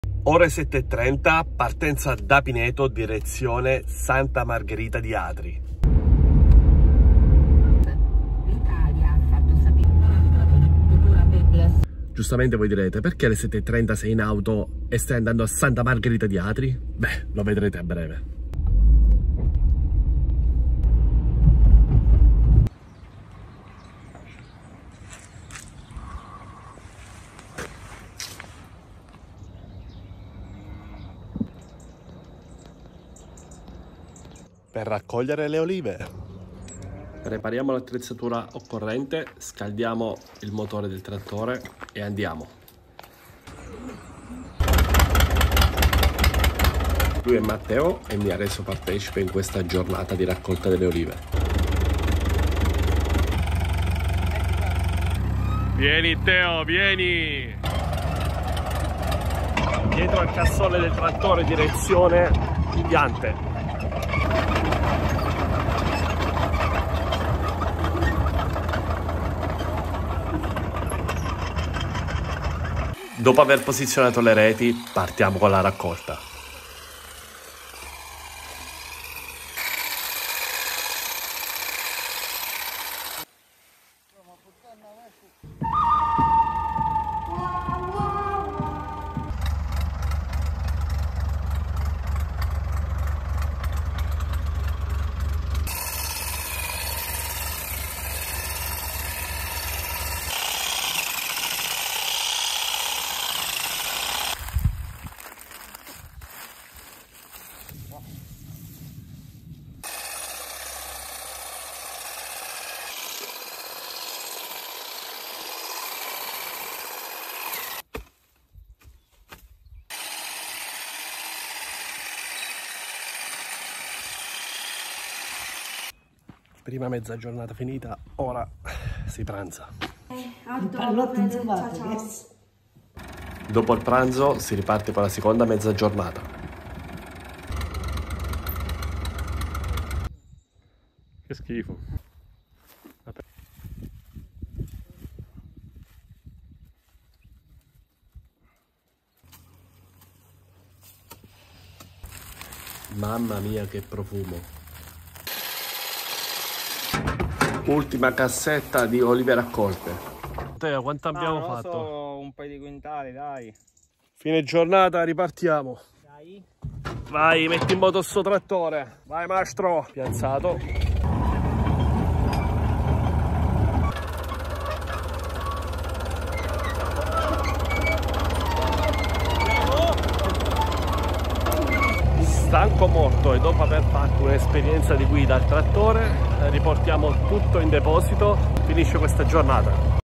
Una giornata da AGRICOLTORE
Torno con un nuovo video sotto un'altra veste! Questa volta vi porto con me in una giornata di raccolta delle olive nelle colline atriane.

A te quand t'ha fatt la liv?